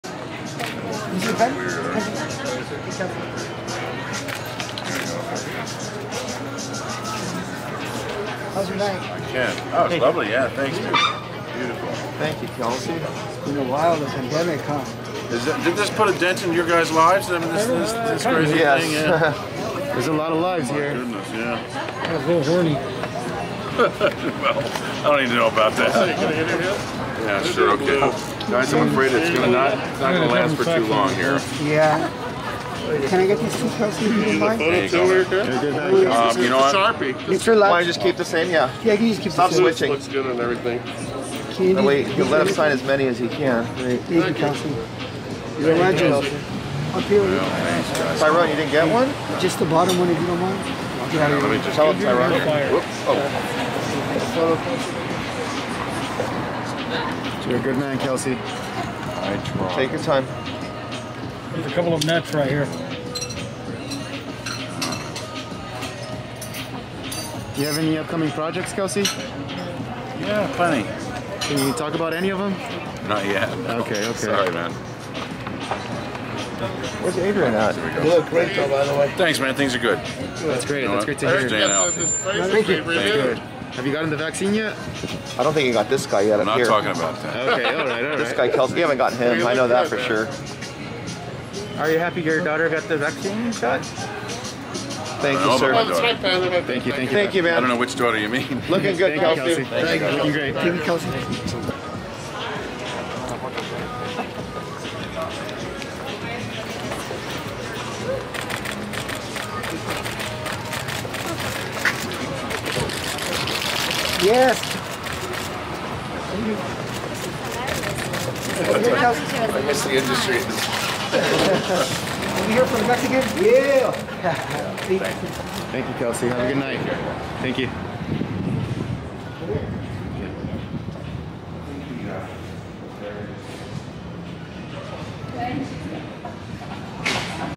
Frasier superstar and actor Kelsey Grammer signs autographs for fans and talks COVID 19 vaccine
Frasier superstar and actor Kelsey Grammer signs autographs for fans and talks COVID 19 vaccine. Kelsey was having dinner with wife Kayte Walsh at Beverly Hills restaurant E. Baldi
4.1.2021